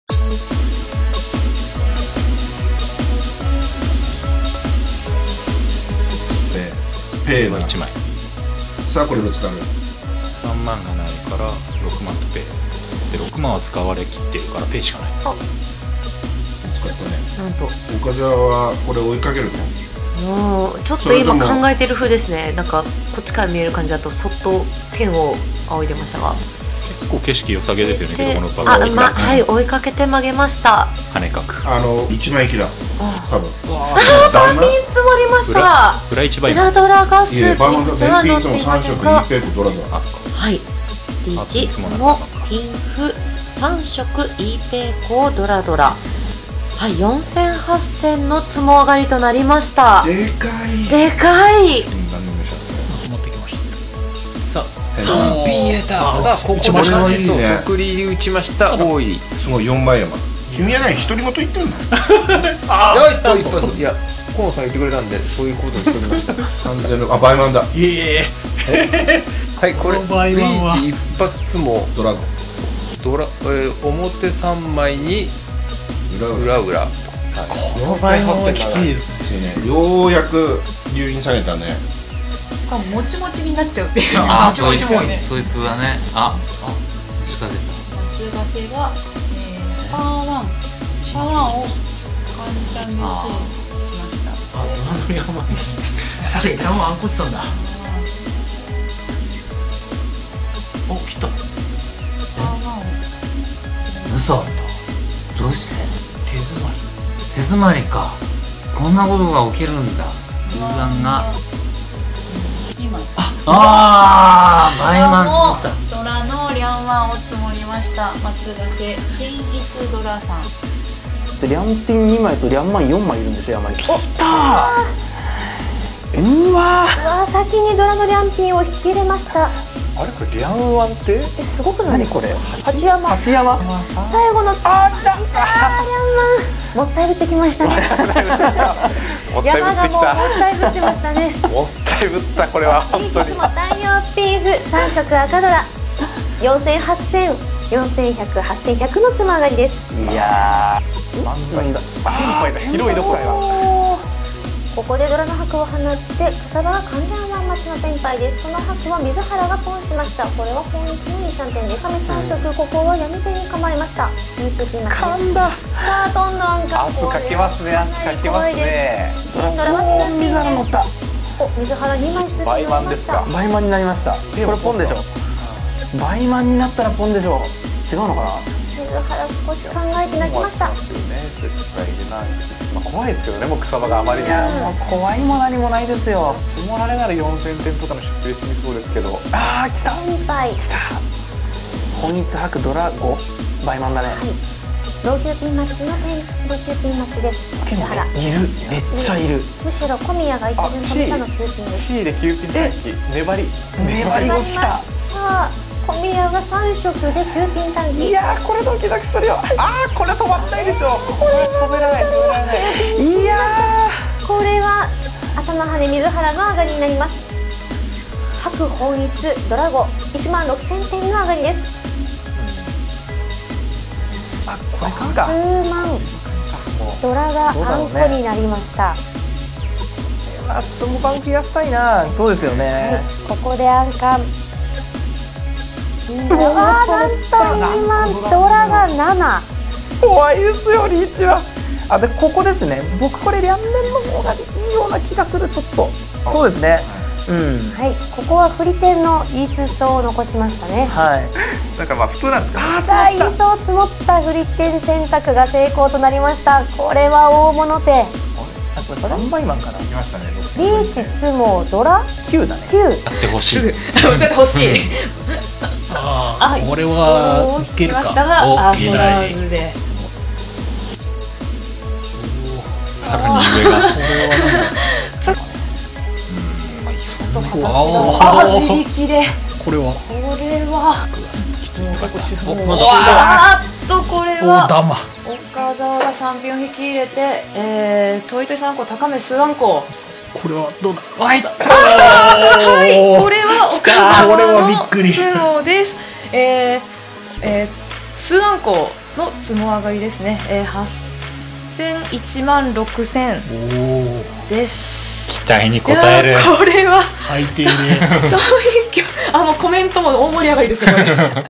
【麻雀】神アガリ厳選集vol.4【2018年6月～7月】
「麻雀スリアロチャンネル」で放映された麻雀対局番組から、倍満以上のアガリをピックアップ！　毎月第2水曜に好評放送中の「スリアロ情報局」でお届けてしている動画を、youtubeでも公開！　麻雀プロたちの強烈なアガリの数々をご堪能ください！